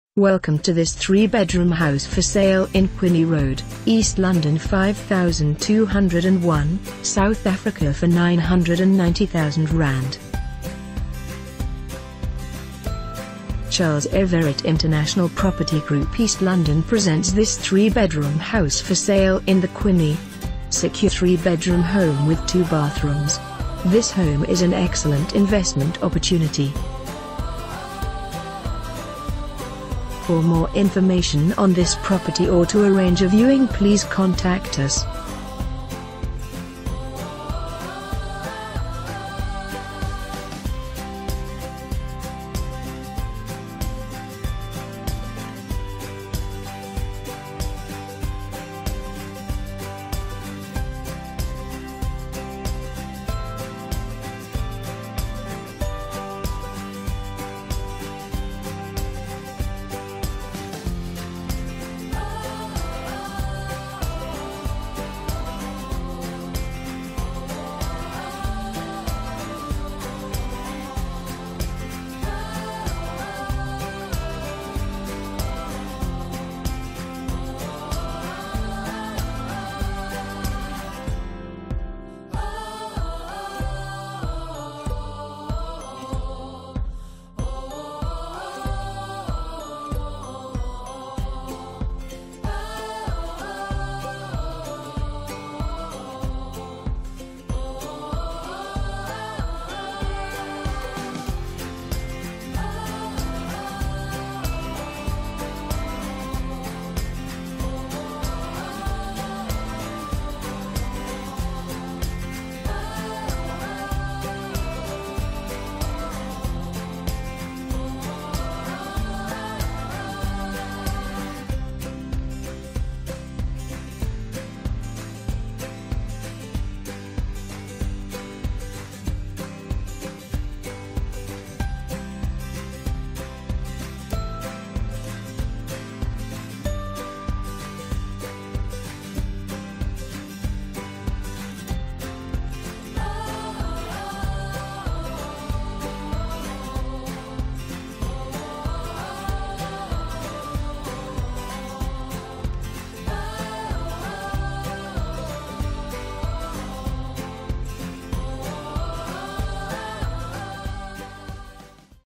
3 Bedroom House For Sale in Quigney Road, East London 5201, South Africa for ZAR 990,000...
To view videos of similar listings in Quigney, East London, Eastern Cape please

Property Description

Chas Everitt International Property Group East London Presents this three bedroom house for sale in the Quigney.  Secure three bedroom home with two bathrooms.  This home is an excellent investment opportunity.

Name: Kerri De la Querra